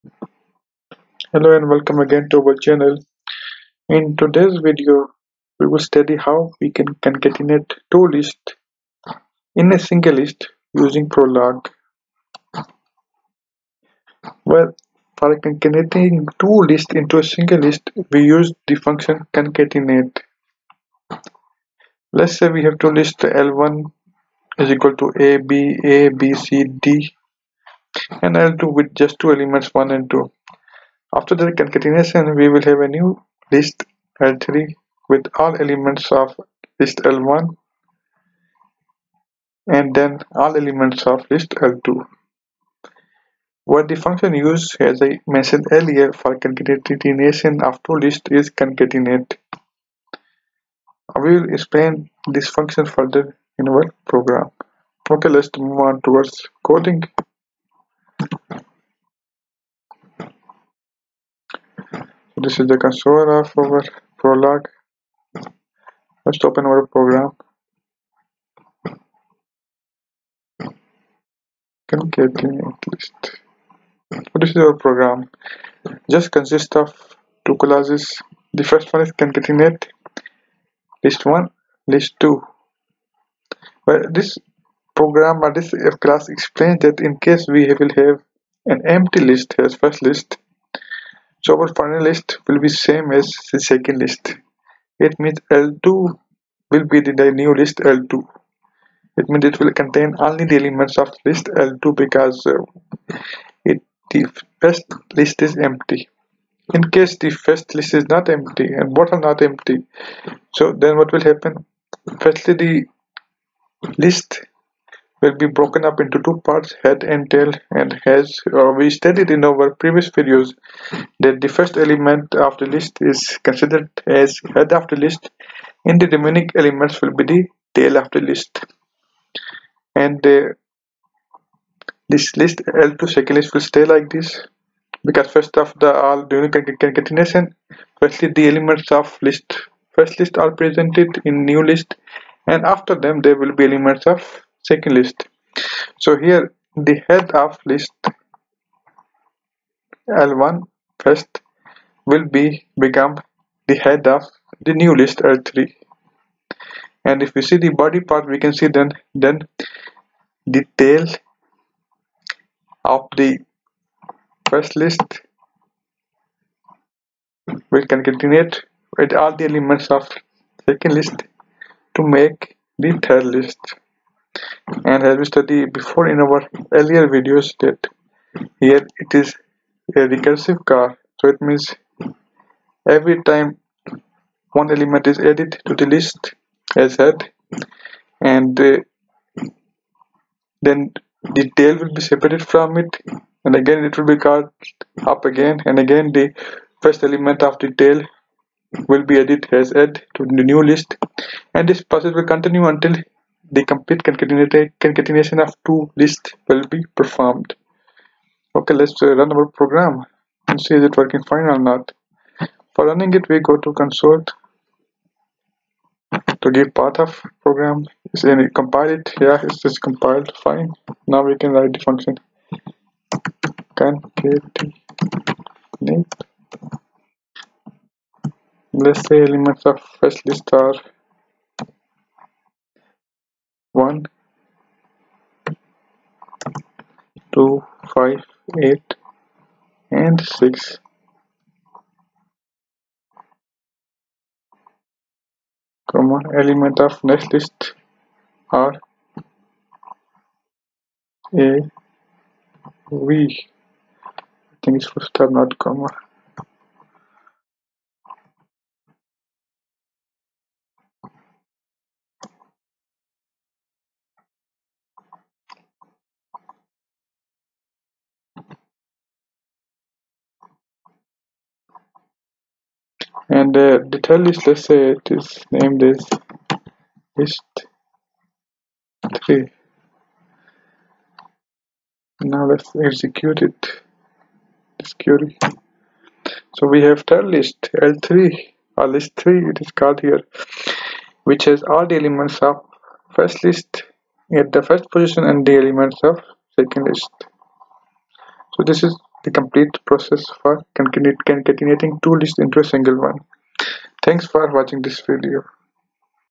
How to Concatenate Lists?
This video explain the coding procedure to concatenate of append two lists into a single list using prolog programming.